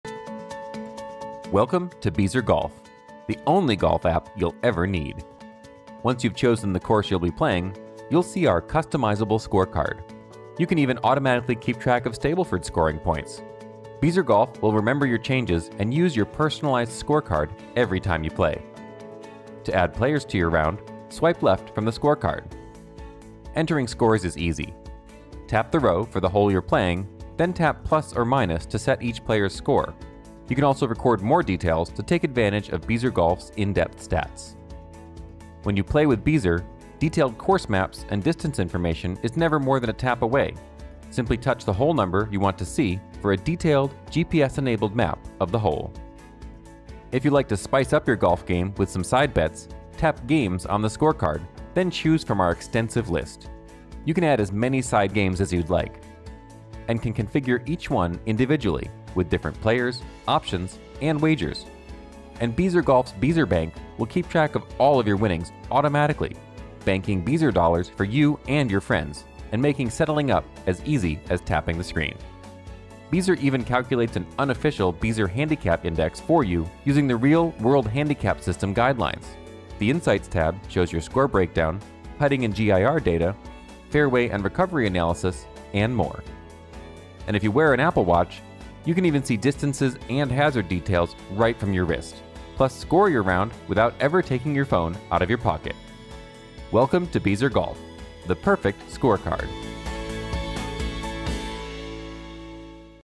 Beezer Golf - Overview - In-App Video
We created a series of short tutorial videos that play in the Beezer Golf App to help users understand how to use all of the features.